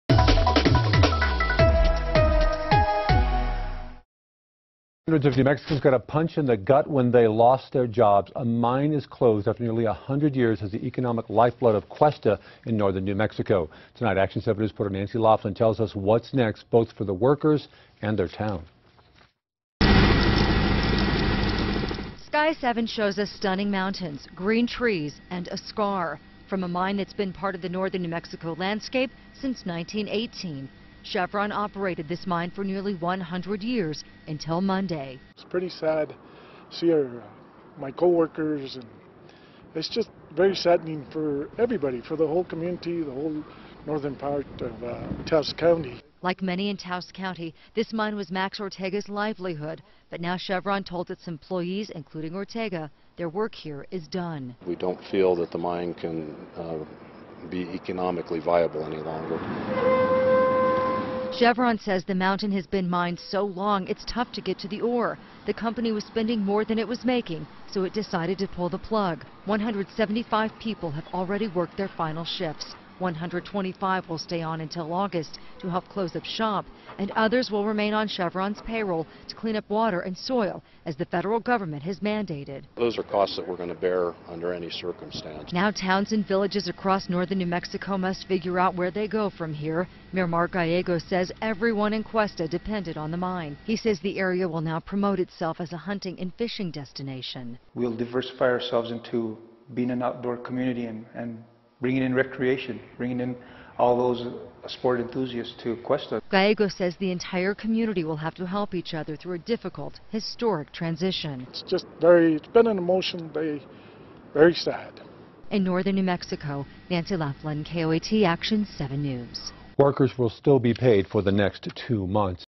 Questa Mine Closing
Today hundreds of New Mexicans got a punch in the gut when they lost their jobs.